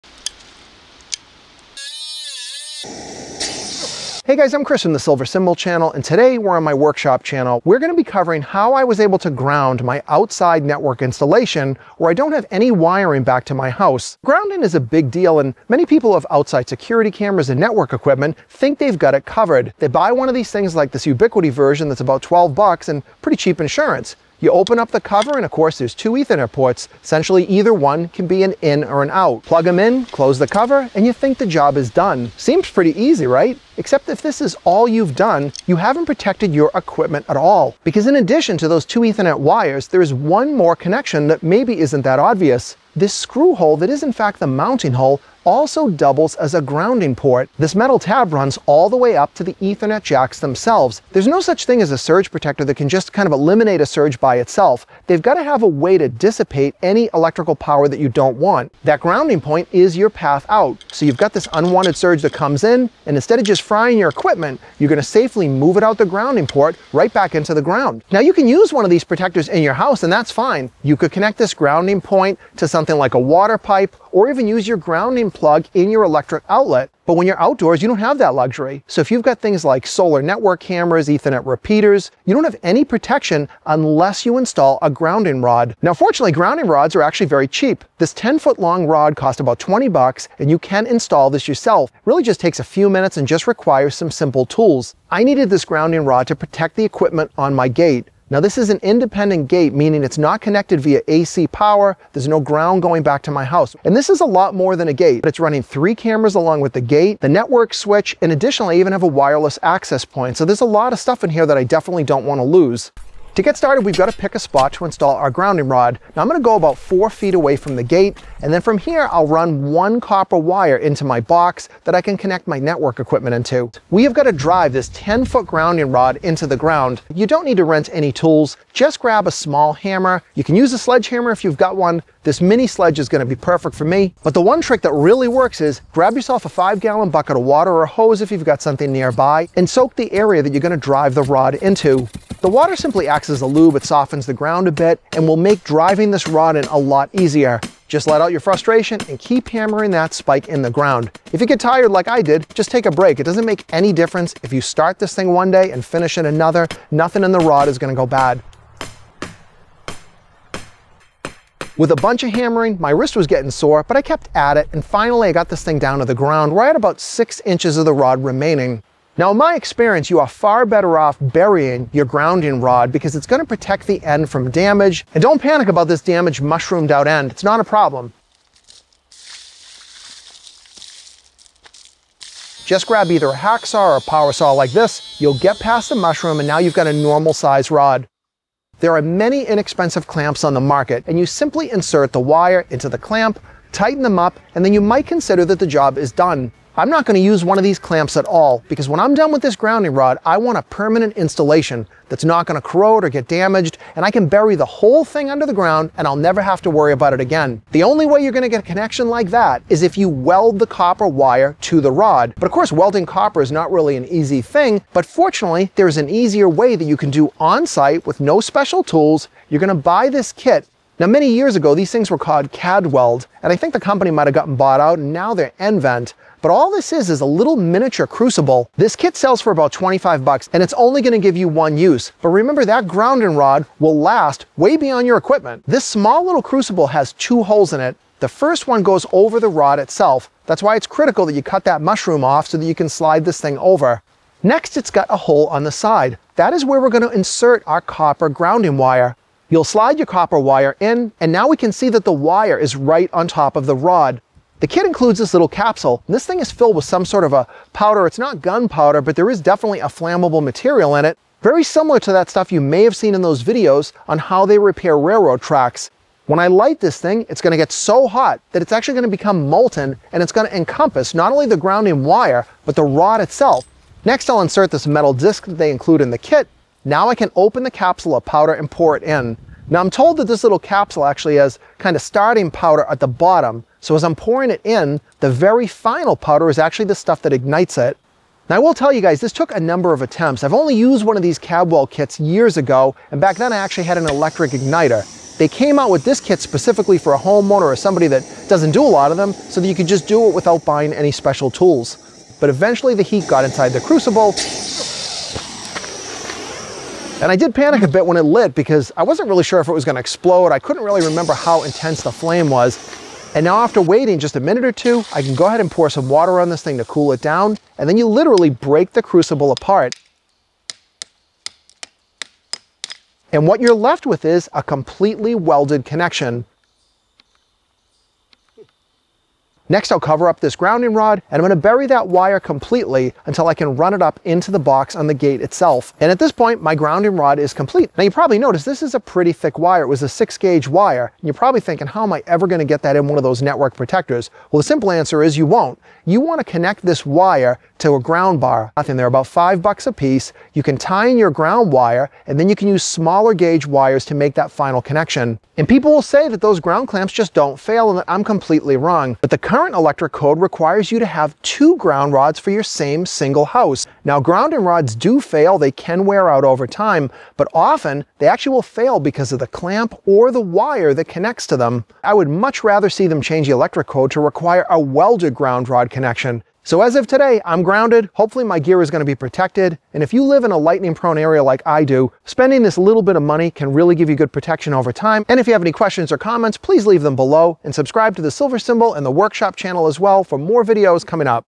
Best Way to Protect Outside Network Equipment From Surges
Protecting outside security camers & network gear can be complicated and when they are solar powered without a ground point, What do you do?  I show you how I installed a ground rod to protect my outdoor Ubuiti cameras, access point & network switches without any connection to my house.

Ground Wire for 120V Outlets
Bus Bar